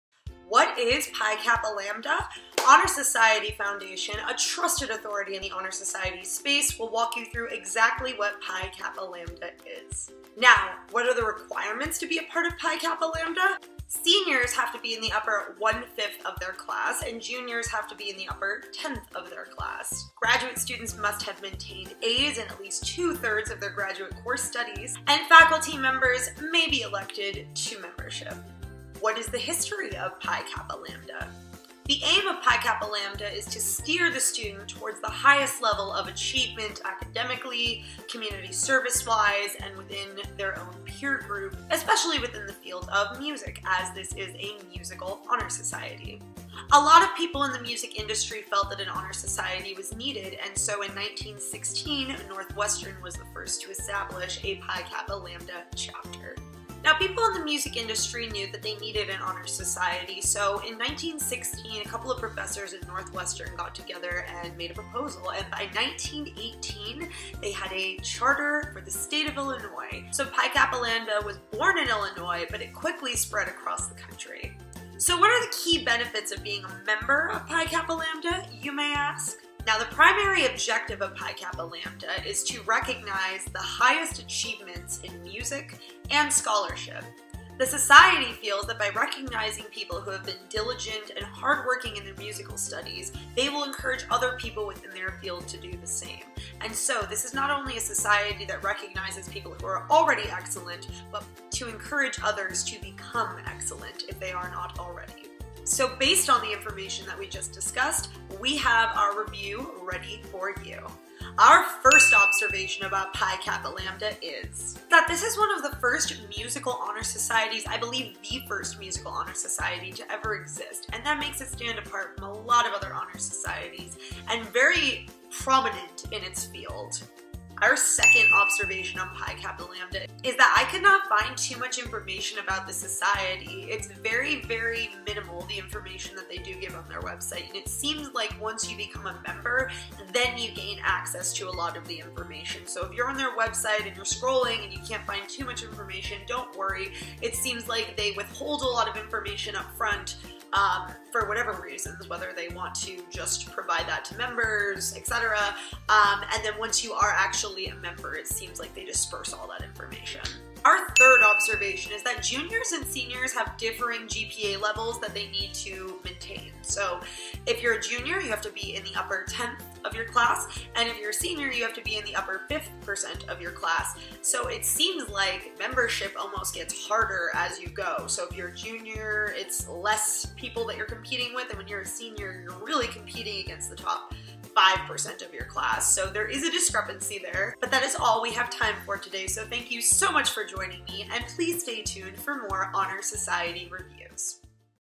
Pi Kappa Lambda Review and Requirements
The Alumni Association of the School of Music at Northwestern University in 1916 appointed a committee to consider the feasibility of installing an honor society in the field of music. Serving on the committee were Carl Milton Beecher, Louis Norton Dodge, and Walter Allen Stults. As a result of their labors, Pi Kappa Lambda was granted a charter by the State of Illinois in 1918.

Check out Honor Society’s “How To Land Your Dream Internship” eBook